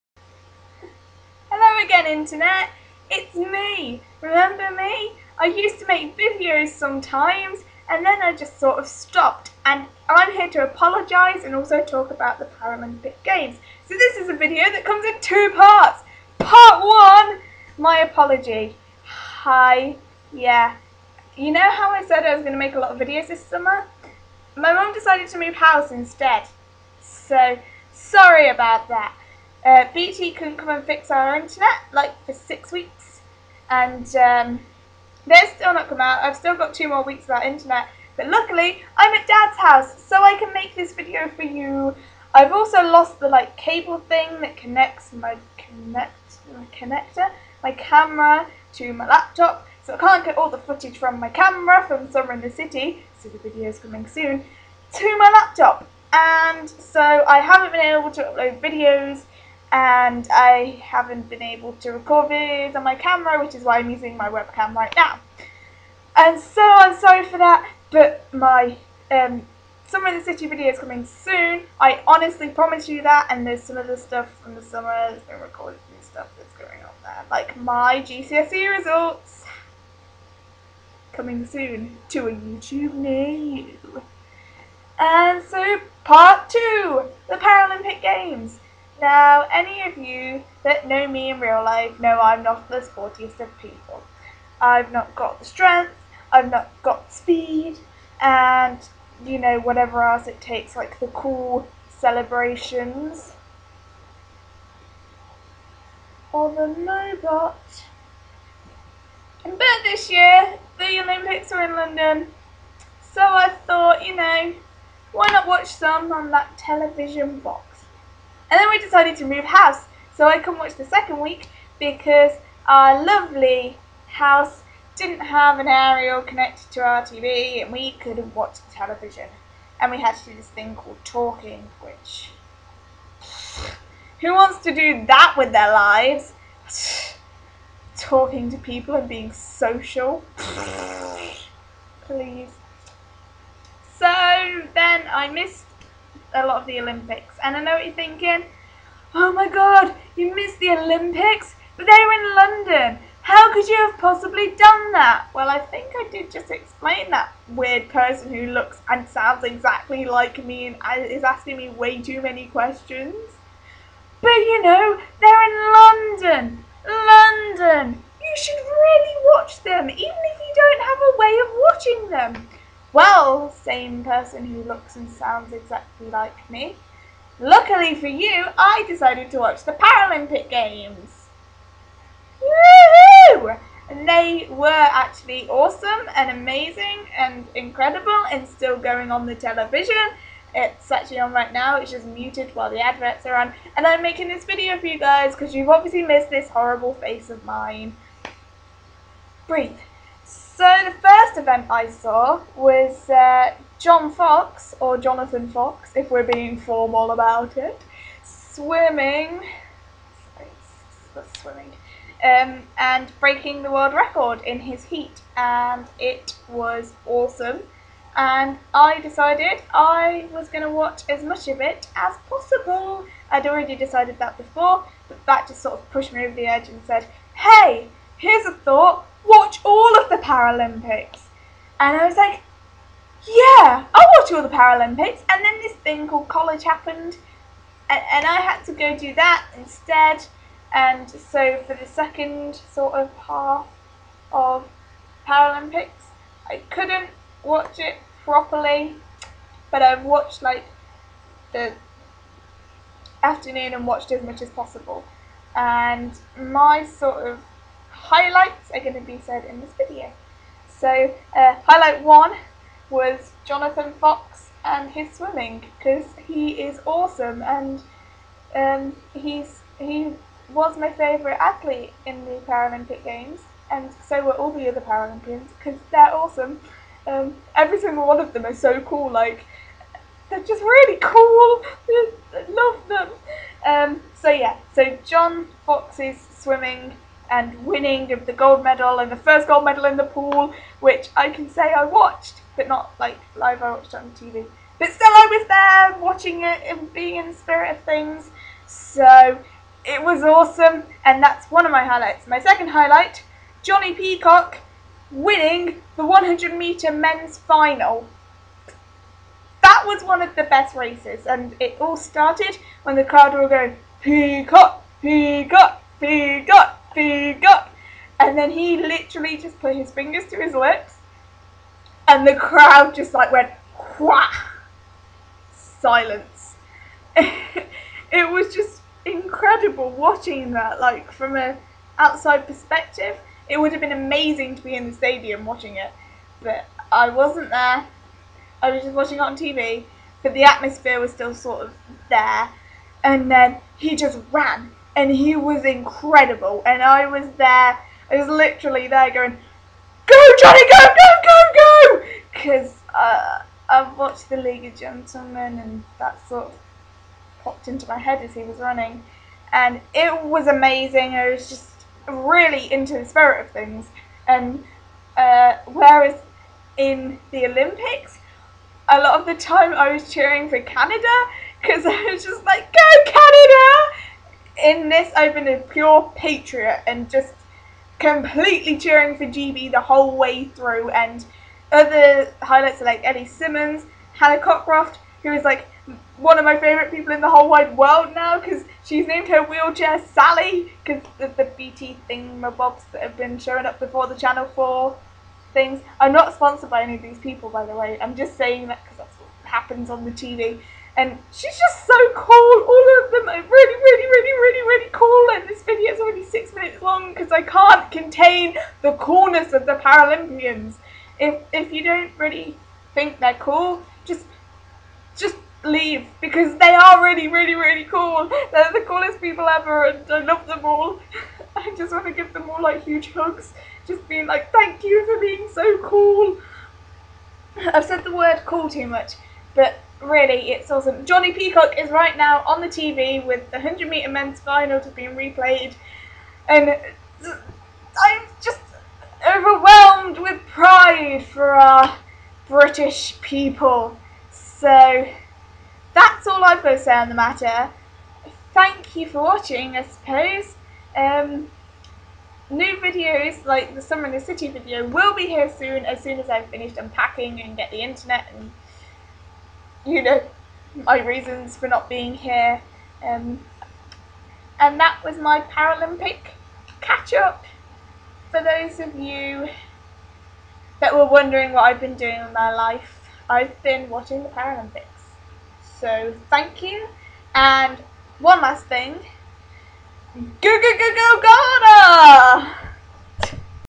The paralympics are cool.
I tell you how cool the paralympians are and apologise for not making videos